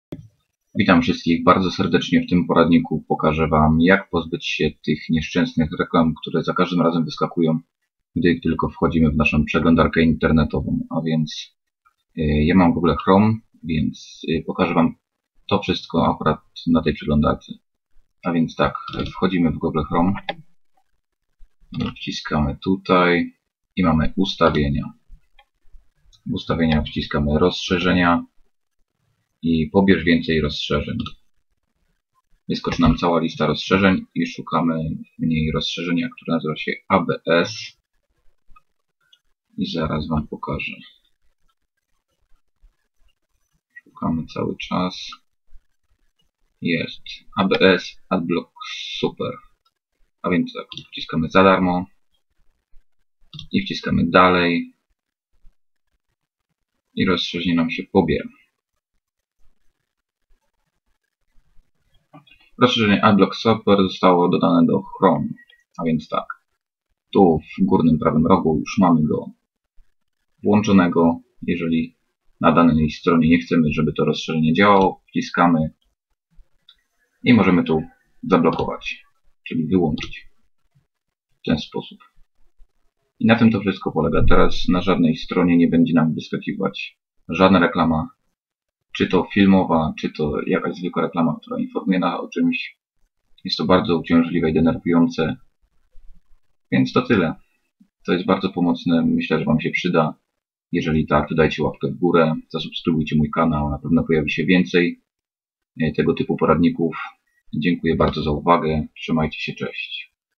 Blokowanie Reklam w Przeglądarce Google Chrome
W tym poradniku pokaże wam jak zablokować reklamy w przeglądarce Google Chrome .
Sposób jest bardzo prosty więc na pewno każdy z was sobie z tym poradzi :)
Jeżeli się poradnik się przydał to liczę na suby/komy i łapki w górę :)